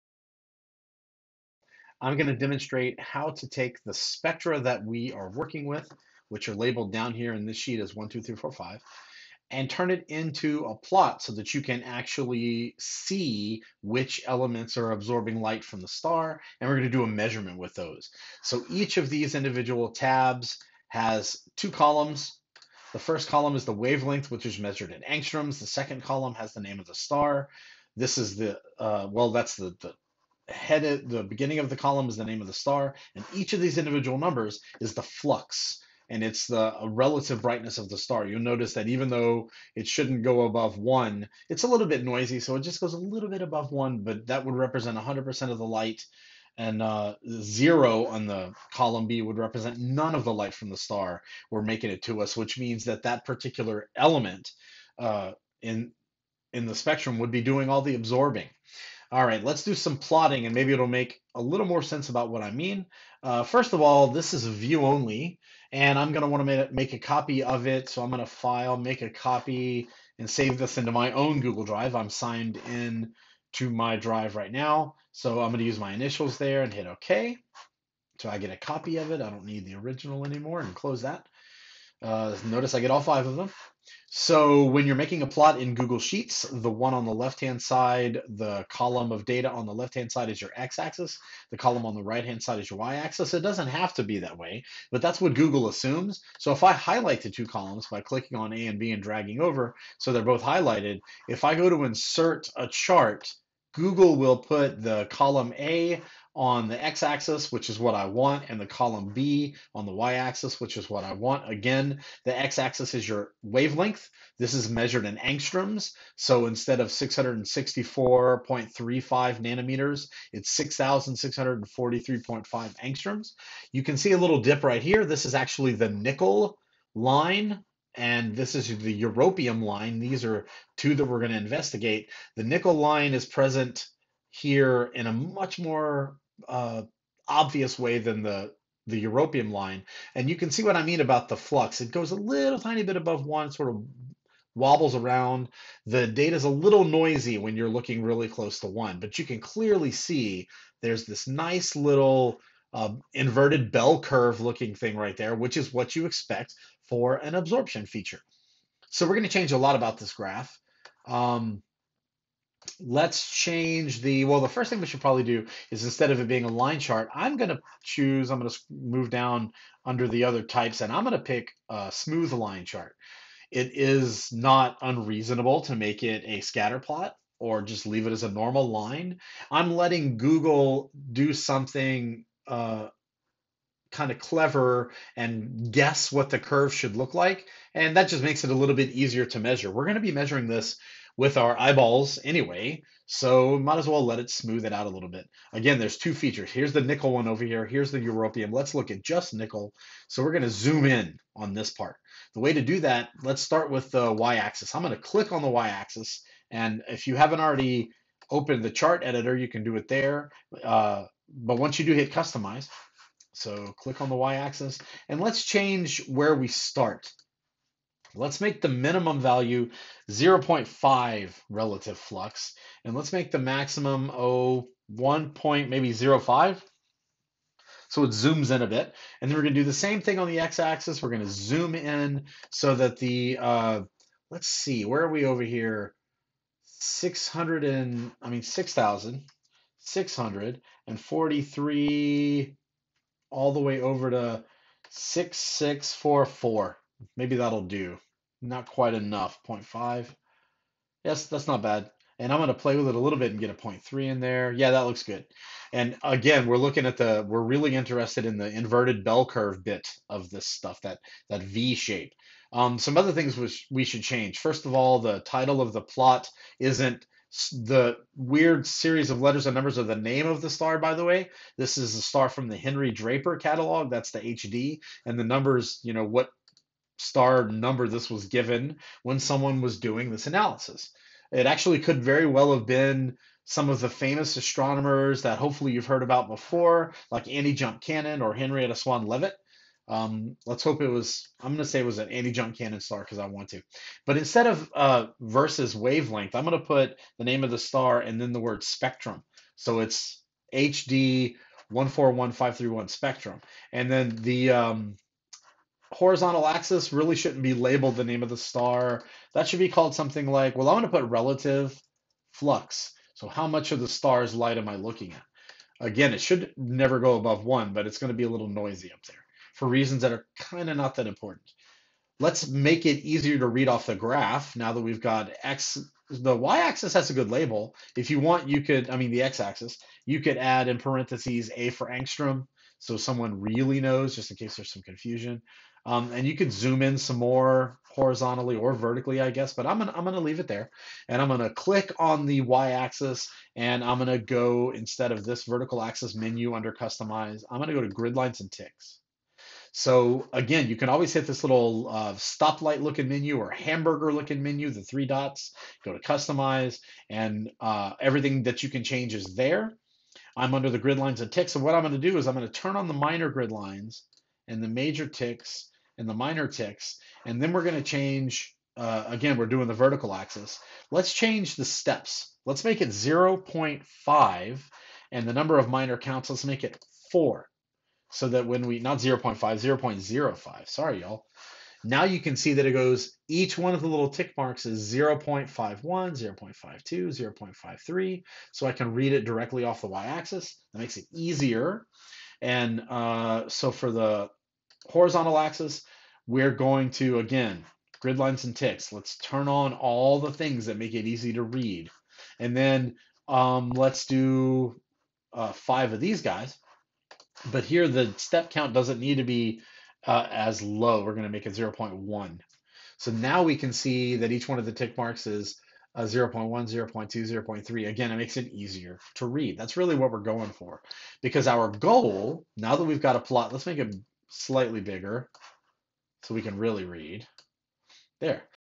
Plotting an Absorption Spectrum
How to setup a plot of an absorption spectrum for easier measurement.